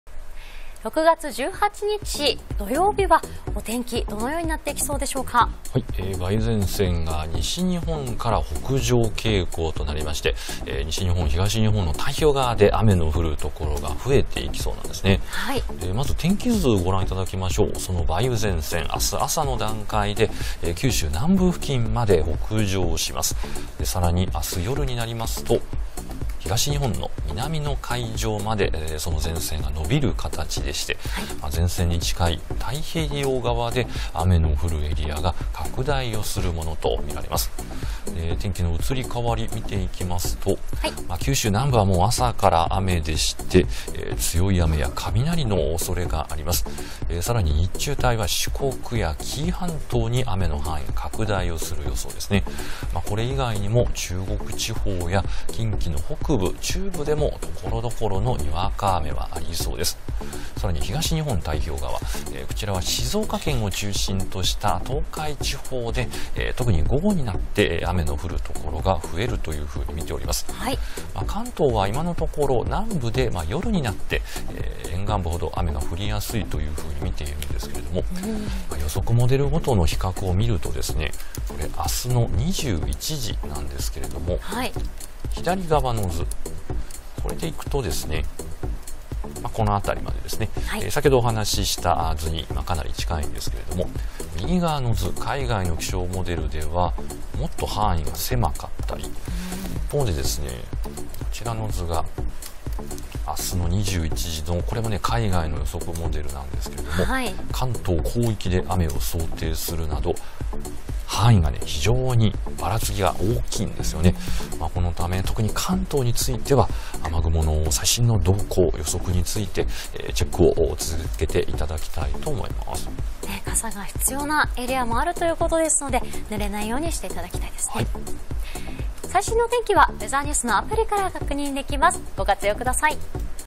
明日は太平洋側で雨 関東も夜遅く雨に
18日(土)は梅雨前線が少し北上し、西日本の太平洋側を中心に雨の強まるおそれがあります。夕方以降は東海や関東でも雲が増えるため、沿岸部を中心ににわか雨にご注意ください。